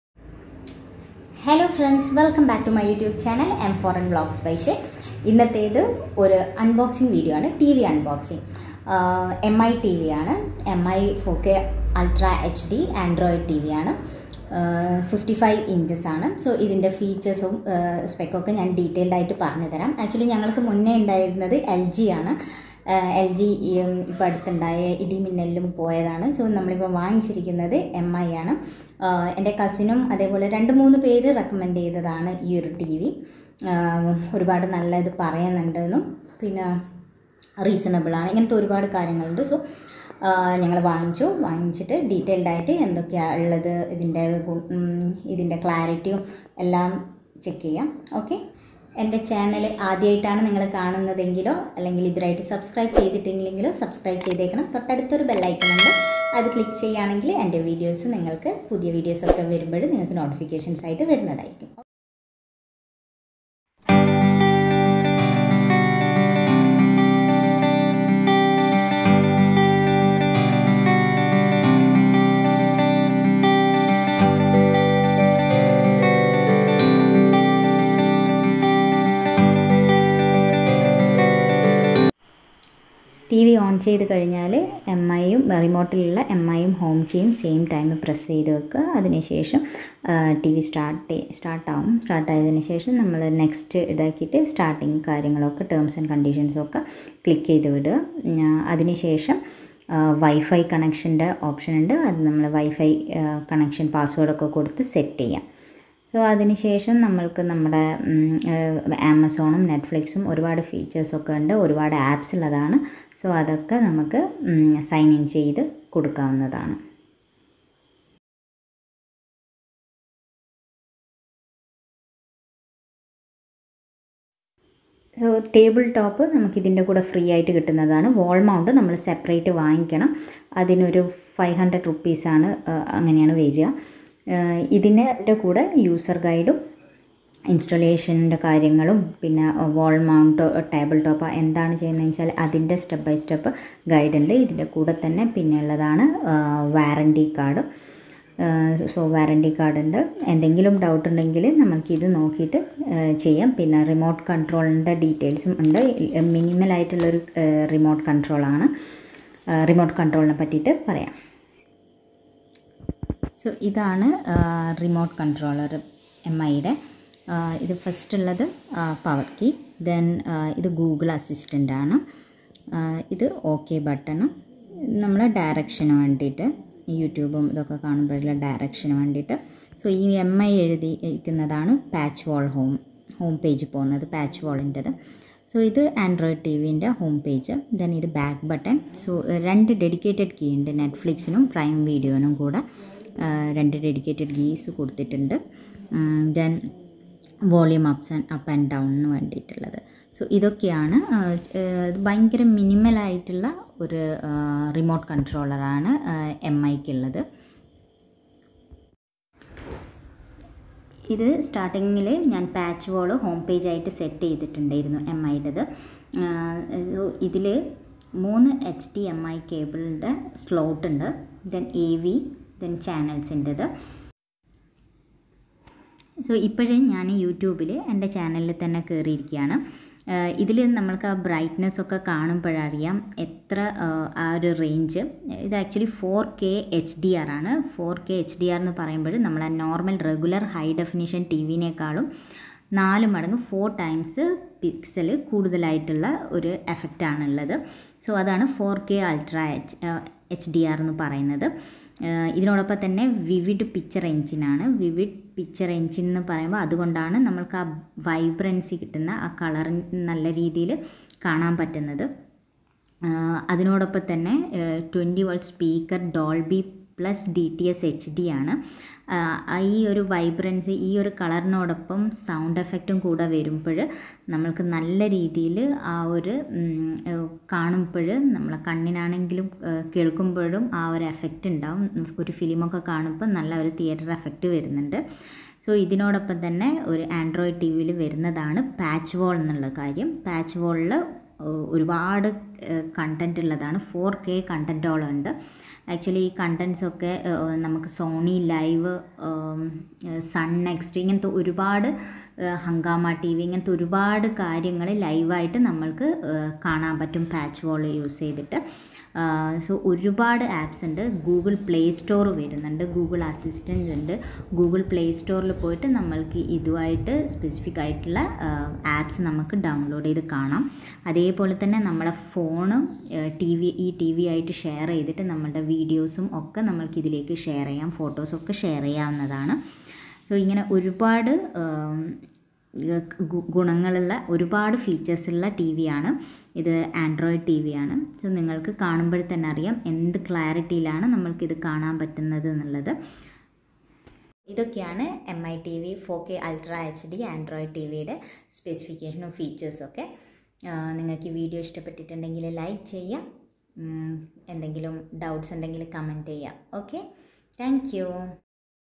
TV Unboxing| MI 4k Ultra HD Android TV| 55 inch| Malayalam | M4L Vlogz by Swe
Mi 4X 4k ultra hd Android TV unboxing with features and specification